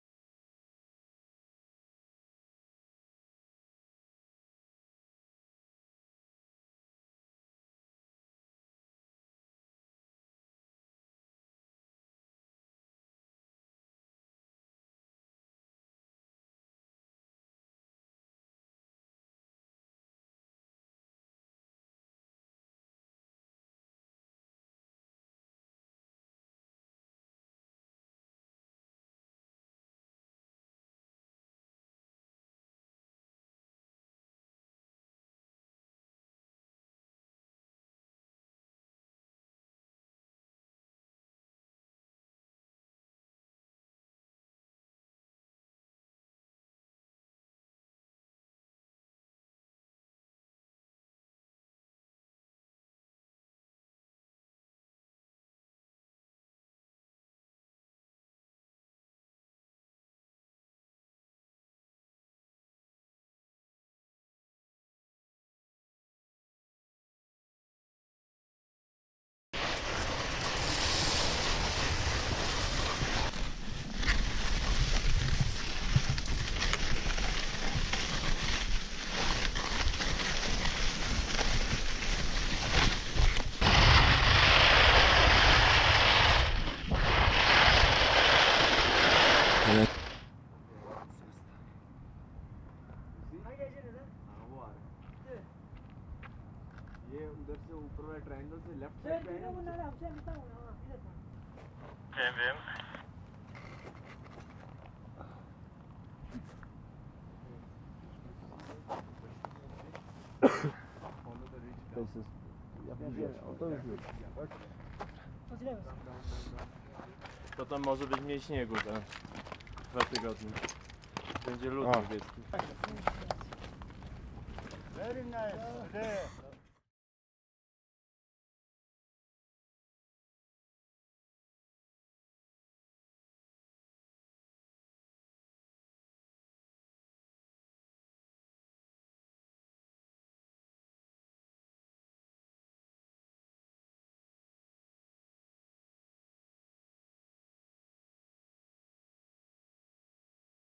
Andrzej Bargiel skis down K2
The Polish ski mountaineer Andrzej Bargiel (30), has become the first person to Ski down K2 (8,611m). It is the second tallest mountain in the world and known as 'the savage mountain'. He summitted K2 at 11:30am on  22nd July 2018 and reached the Base Camp at around 7:30pm local time.  (apologies for the incorrect date in the video!)
Video courtesy Red Bull Content Pool, Andrzej Bargiel and Marek Ogien.
23rd July 2018